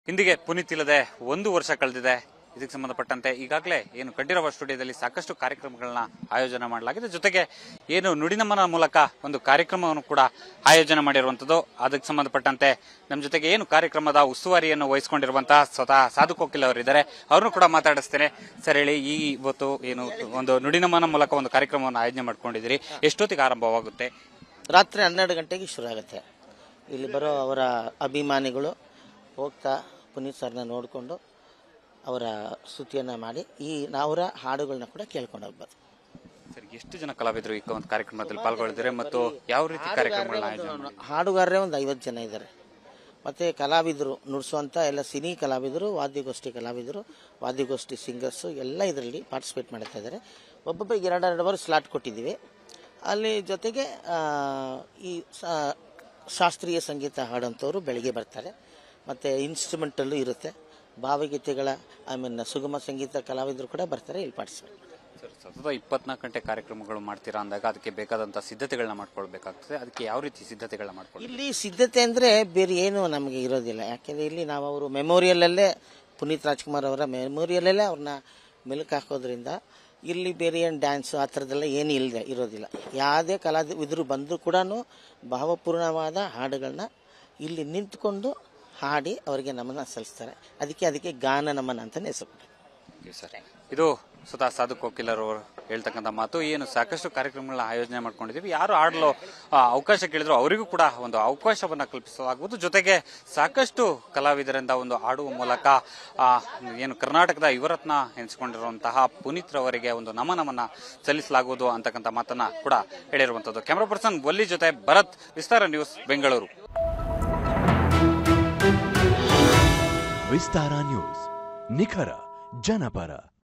Sadhu Kokila: ಅಪ್ಪು ನುಡಿ ನಮನ ಕಾರ್ಯಕ್ರಮಕ್ಕೆ ಹೇಗಿದೆ ತಯಾರಿ? | Vistara News Kannada
ಕಂಠೀರವ ಸ್ಟುಡಿಯೋದಲ್ಲಿ ಹಾಸ್ಯನಟ ಸಾಧುಕೋಕಿಲಾ ನೇತೃತ್ವದಲ್ಲಿ ನುಡಿ ನಮನ ಕಾರ್ಯಕ್ರಮವನ್ನು ಹಮ್ಮಿಕೊಳ್ಳಲಾಗಿದೆ. ಮಧ್ಯಾರಾತ್ರಿ 12 ಗಂಟೆ ಶುರುವಾಗಲಿದ್ದು, ಅದರ ತಯಾರಿಯ ಬಗ್ಗೆ ಸ್ವತಃ ಸಾಧುಕೋಕಿಲಾ ವಿಸ್ತಾರ ನ್ಯೂಸ್​ ಜತೆ ಮಾತನಾಡಿದ್ದಾರೆ.